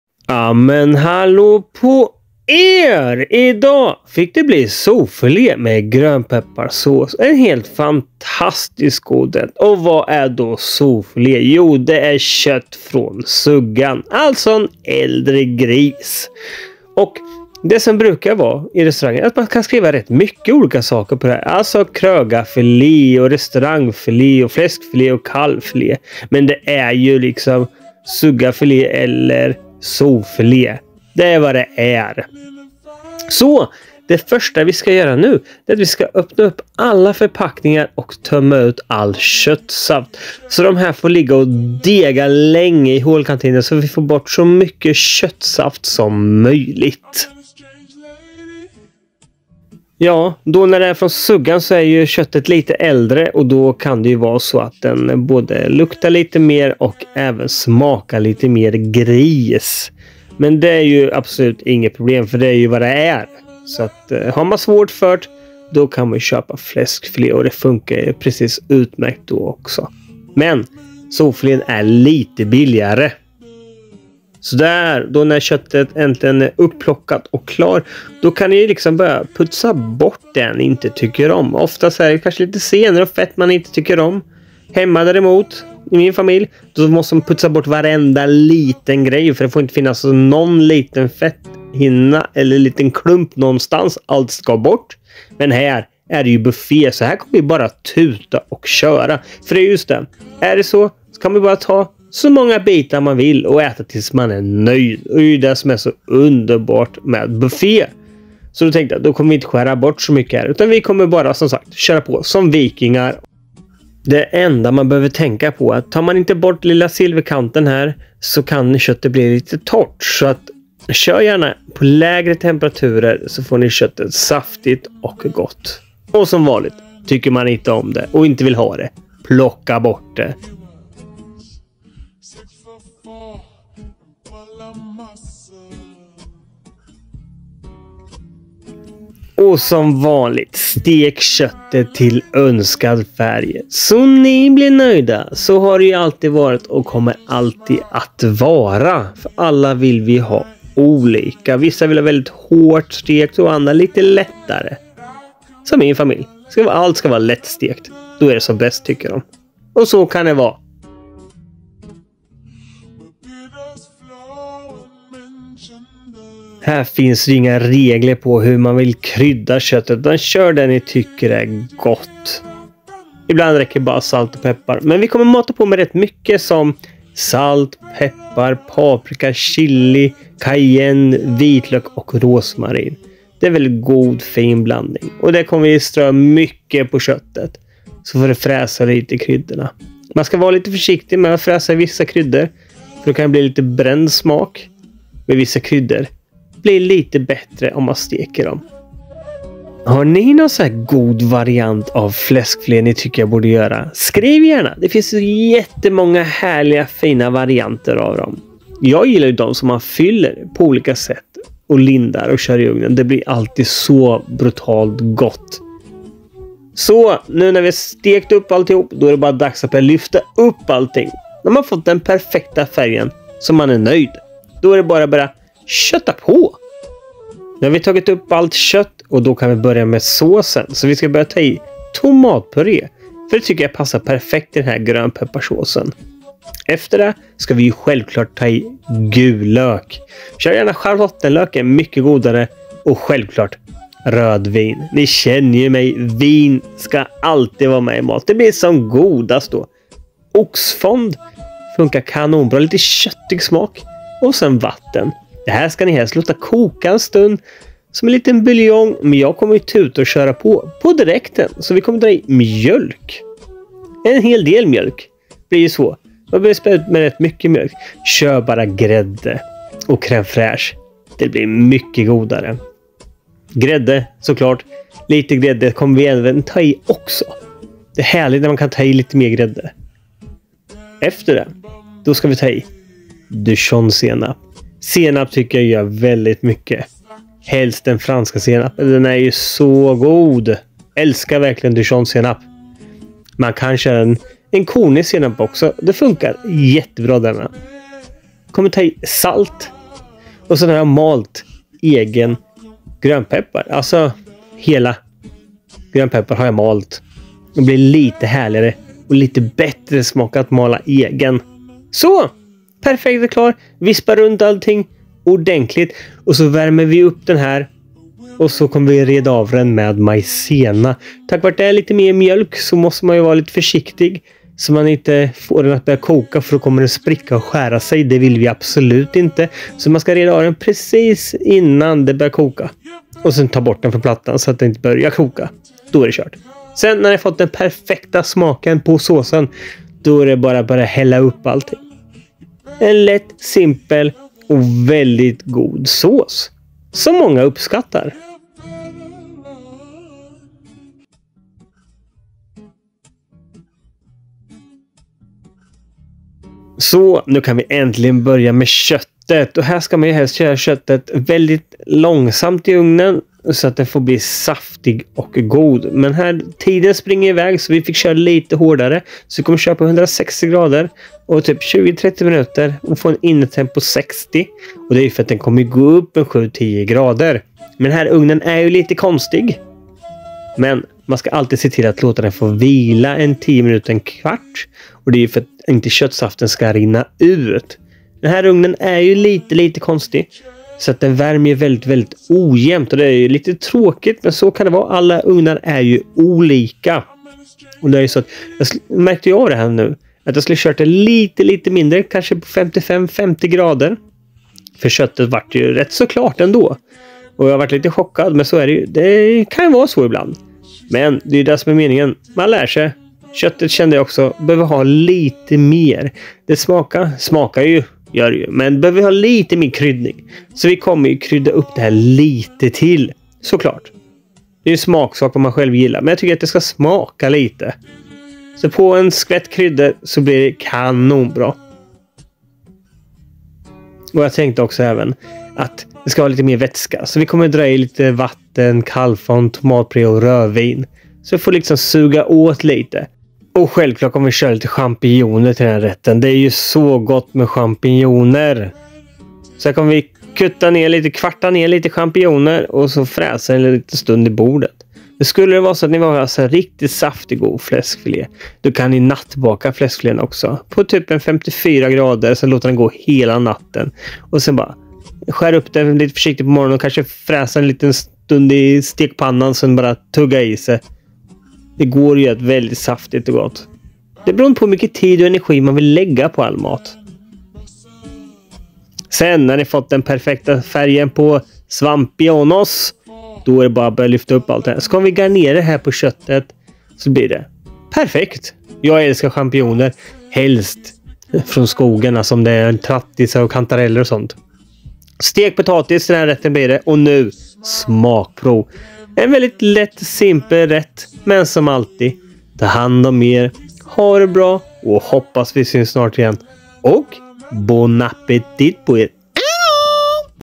Sofilé med grönpepparsås
Här kommer en till matlagningsvideo.
Denna gång blev det Sofilé med grönpepparsås.

Bjud på helstekt Sofilé med en smarrig gräddsås smaksatt med grönpeppar och senap. En rätt som inte tar så långtid att göra men ändå känns god och lyxig. Finns det inte Sofilé så funkar fläskfilé ut märkt också eller något annat kött. Servera med rostad potatis. Garanterad succé!
Smaklig måltid.